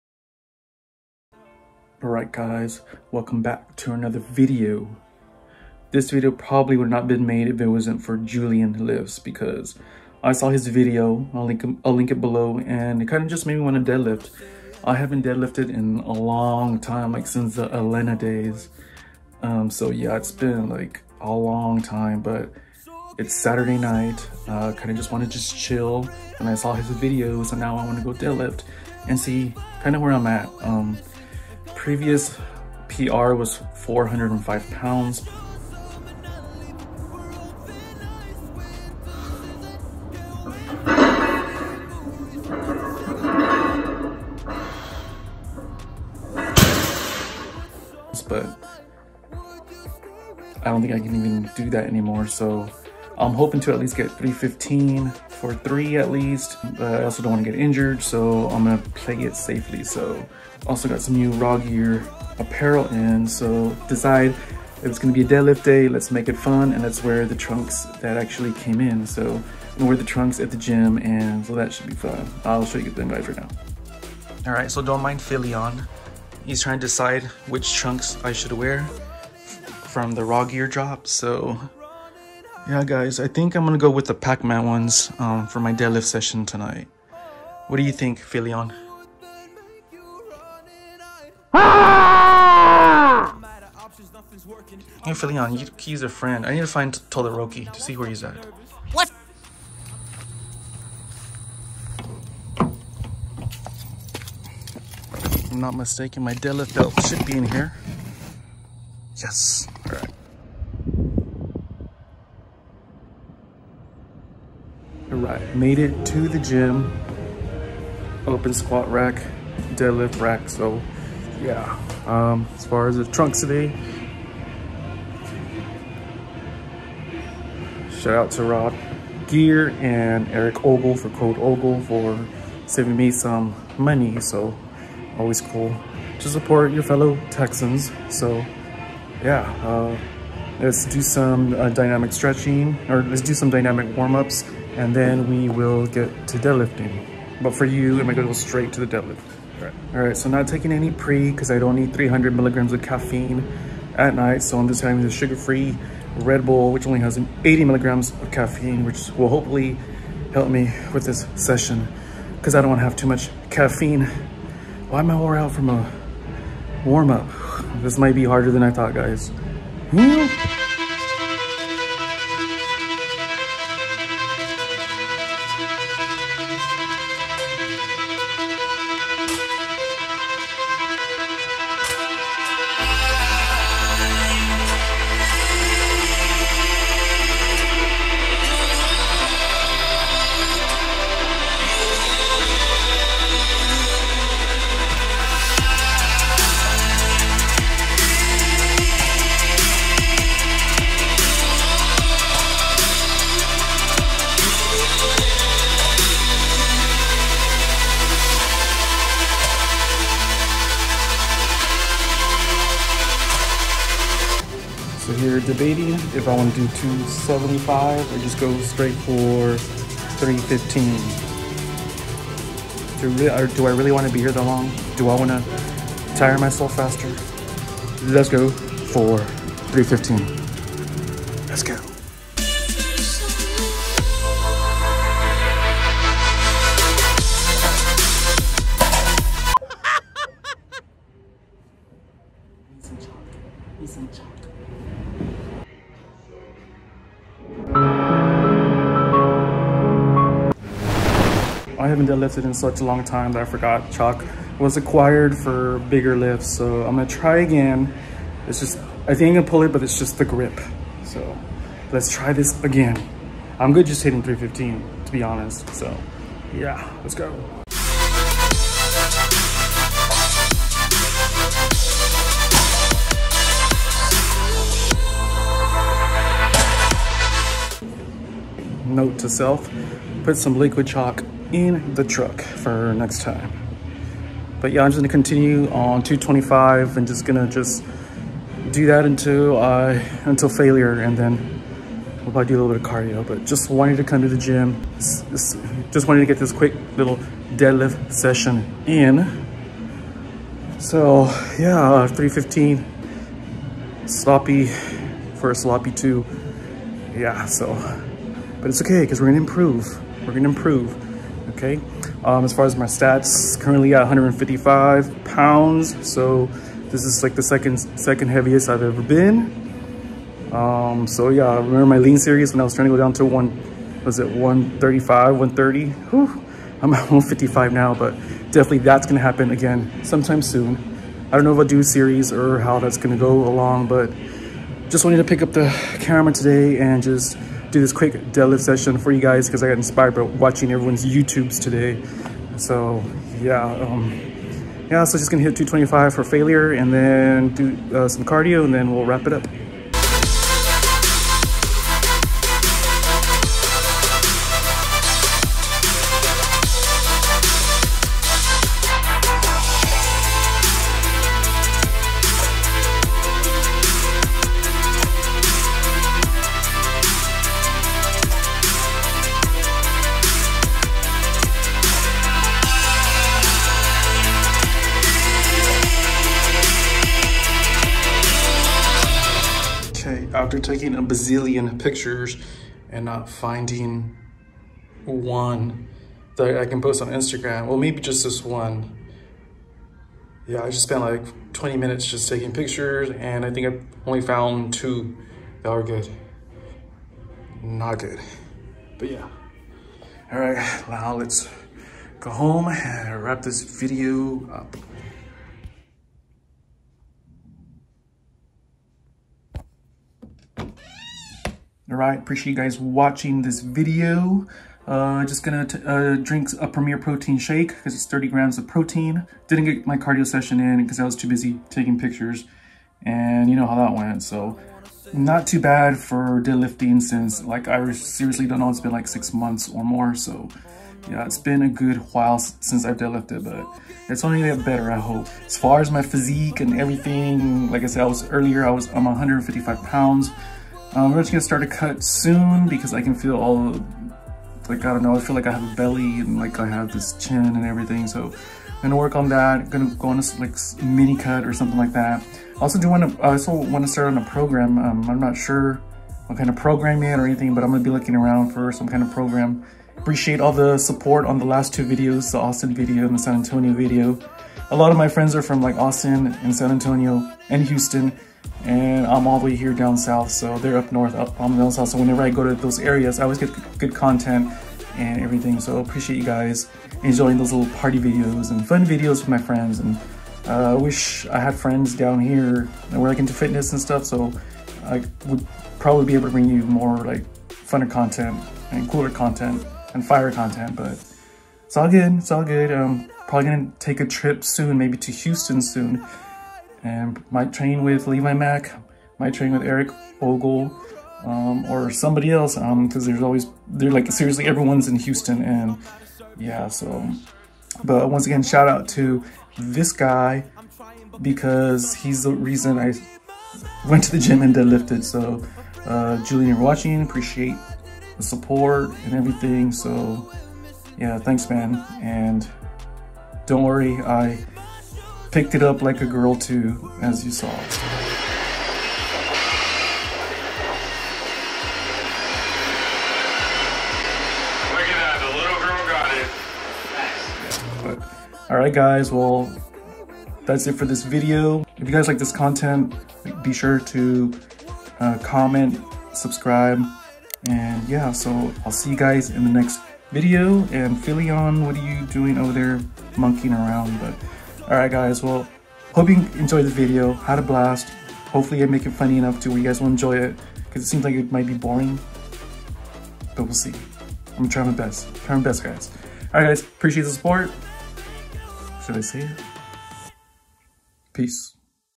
Return to Deadlifts?
Just a random night that I was watching YouTube and got inspired to deadlift.  Mind you it has been many months since I have deadlifted!

Can I make a comeback?

Enjoy this fun video.